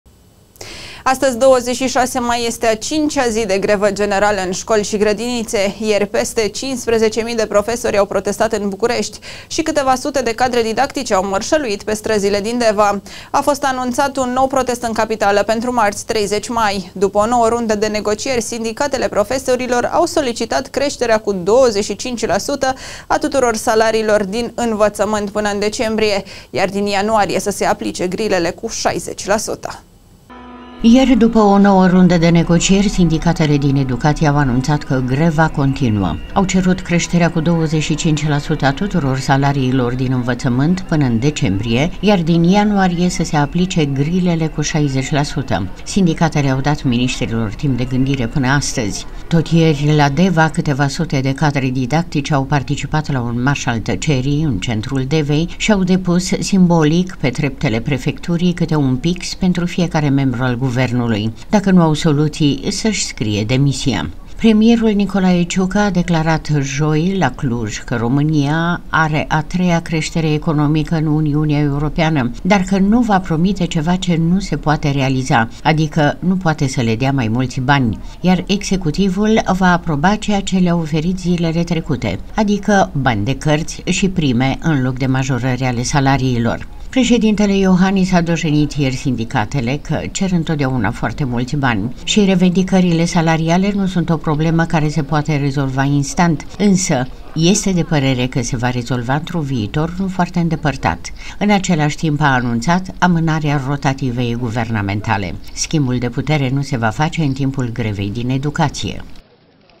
„GREVA ÎN EDUCAȚIE CONTINUĂ!”A FOST ANUNȚAT UN NOU PROTEST ÎN CAPITALĂ PENTRU MARȚI, 30 MAI
„GREVA ÎN EDUCAȚIE CONTINUĂ!”
A FOST ANUNȚAT UN NOU PROTEST ÎN CAPITALĂ PENTRU MARȚI, 30 MAI
         Astăzi, 26 mai, este a cincea zi de grevă generală în școli și grădinițe. Ieri, peste 15.000 de profesori au protestat în București, și câteva sute de cadre didactice au mărșăluit pe străzile din Deva. A fost anunțat un nou protest în Capitală pentru marți, 30 mai. După o nouă rundă de negocieri, sindicatele profesorilor au solicitat creșterea cu 25% a tuturor salariilor din Învățământ până în decembrie, iar din ianuarie să se aplice grilele cu 60%.